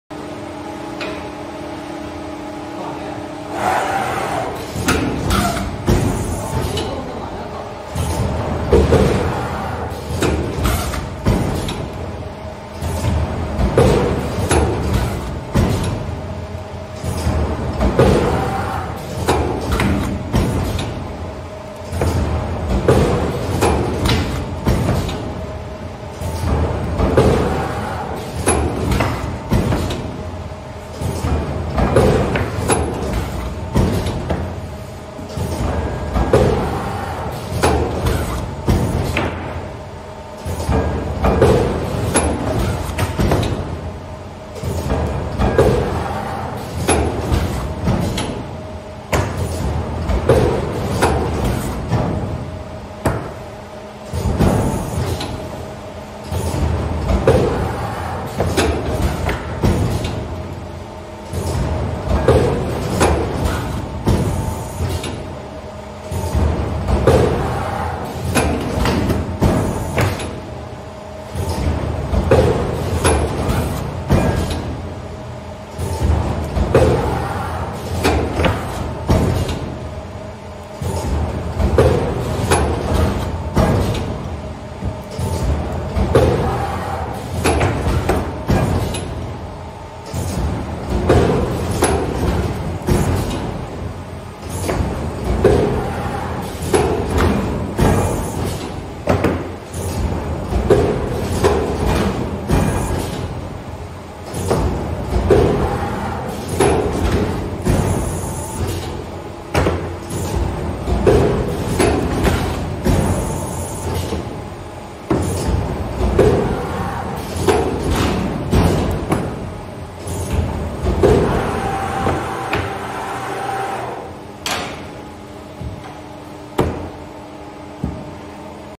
TM-1080D Automatic Double Heads Waste Paper Stripping Machine
MSTM-1080D Automatic Double Heads Waste Paper Blanking Machine for Paper Cup

Machine suitable for same size but different opposite direction product on one sheet.